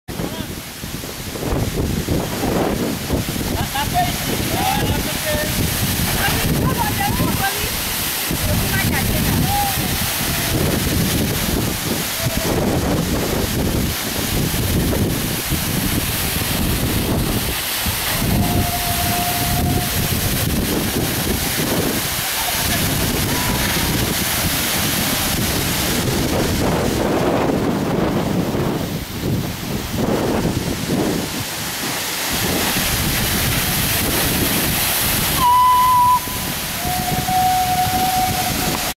vos vacances au Togo avec cascade de Yikpa -Togo (suite)
En pleine baignade dans la cascade de Yikpa